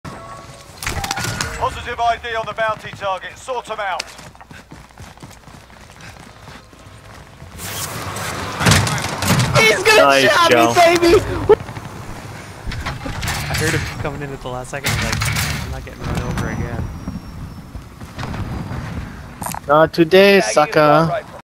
not this time!
For once I didn't get ran over/chopped up by a Heli! Dang it feels good to be a gangsta! Dude I killed even had some fun with it too